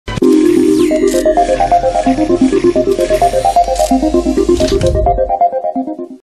ID de LS 83 TV Canal 9 Buenos Aires - 2008
Nuevo ID del canal de aire dedicado casi en su totalidad a la emisión de contenido envasado extranjero. Se podría decir que la cinta que se ve en este clip es una clara alusión a la programación del canal.